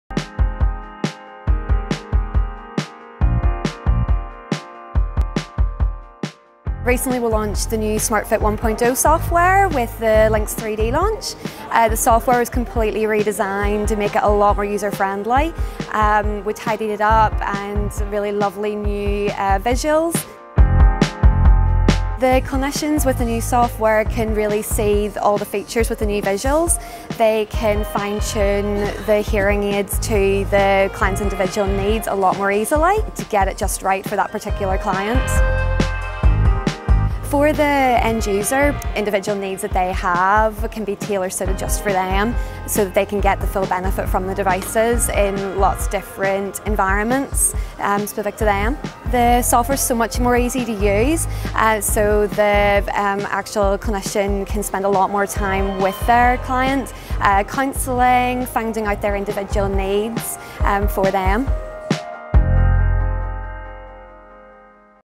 ReSound Smart Fit - enhanced fitting software
Lisa McBride, Audiologist & Product Specialist, NSW & ACT ReSound talks about the new Smart Fit software from ReSound and the benefits to both clients and clinicians.